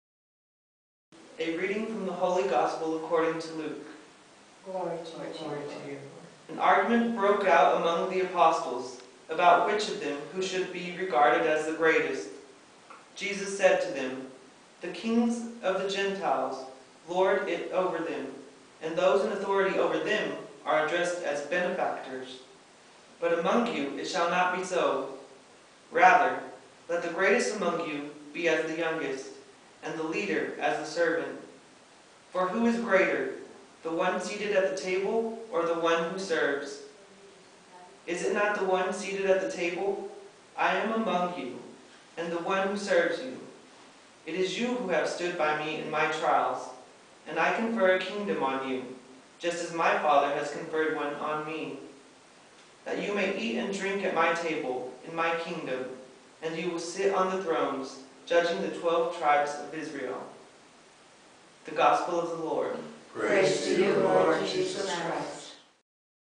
09/03/10 Gospel
Nicholas Flores reads the Gospel for today, Memorial of Saint Gregory the Great, pope and doctor of the Church, from the Guadalupe Chapel in the Catholic Center.